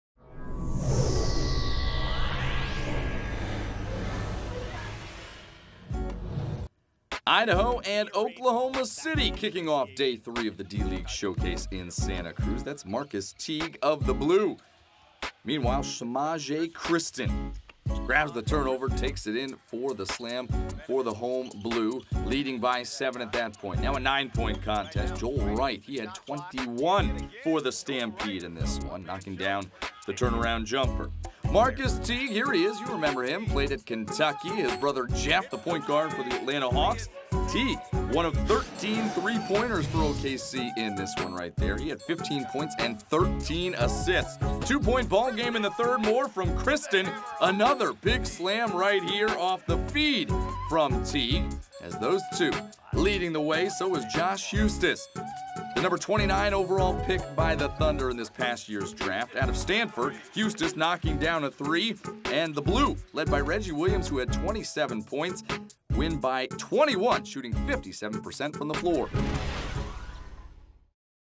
D-League Showcase Game Recap: Stampede vs. Blue
Reggie Williams scored 27 points for the Oklahoma City Blue as they defeated the Idaho Stampede 116-95.